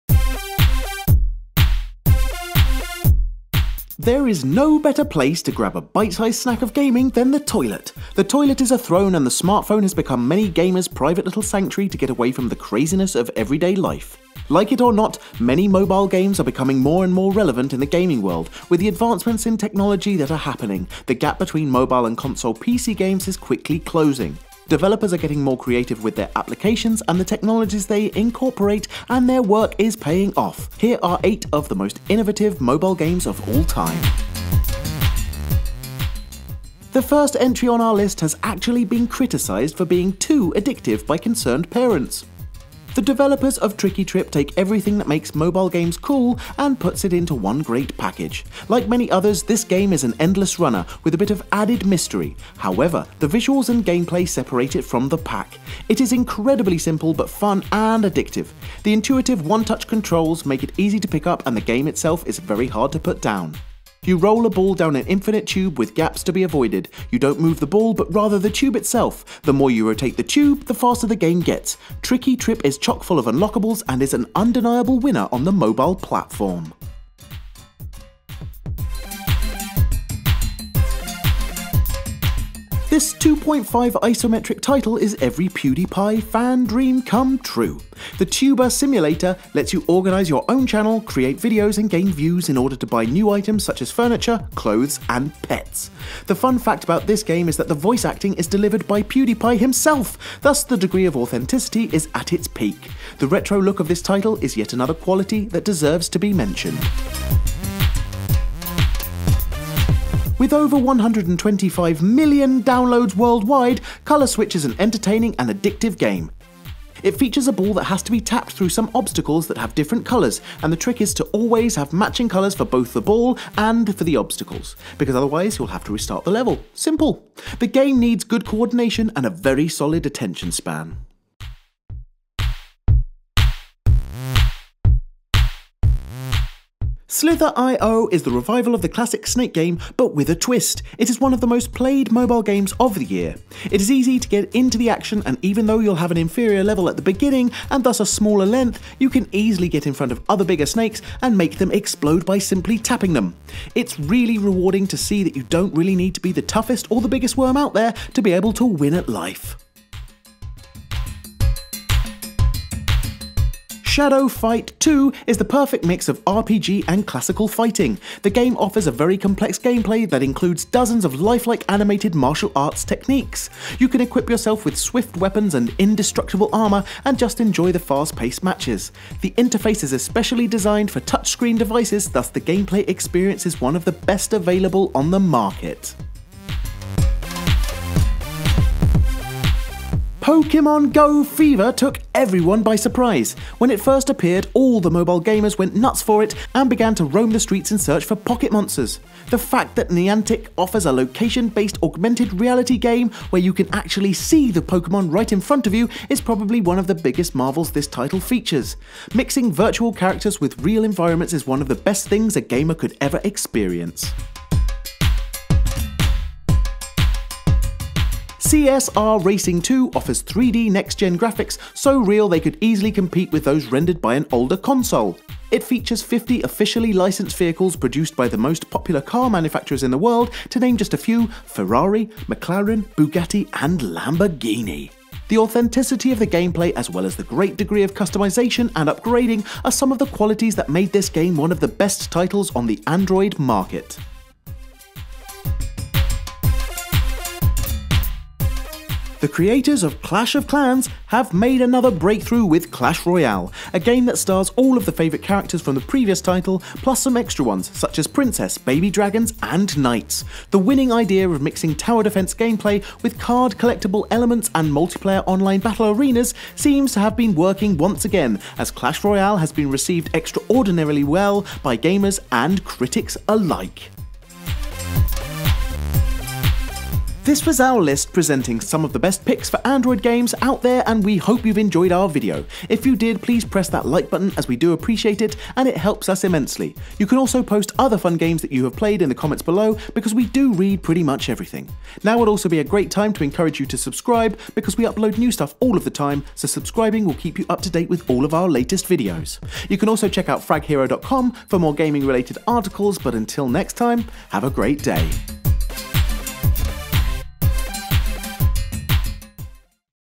8 Most Addictive Android Games That Are Worth Playing
There is no better place to grab a bite sized snack of gaming than the toilet. The toilet is a throne, and the smartphone has become many gamers’ private little sanctuary to get away from the craziness of everyday life. Like it or not, mobile games are becoming more and more relevant in the gaming world. With the advancements in technology that are happening, the gap between mobile and console/PC games is quickly closing. Developers are getting more creative with their applications and the technologies they incorporate and their work is paying off. Here are 8 of the most innovative mobile games of all time.

narrator: Davide Arbis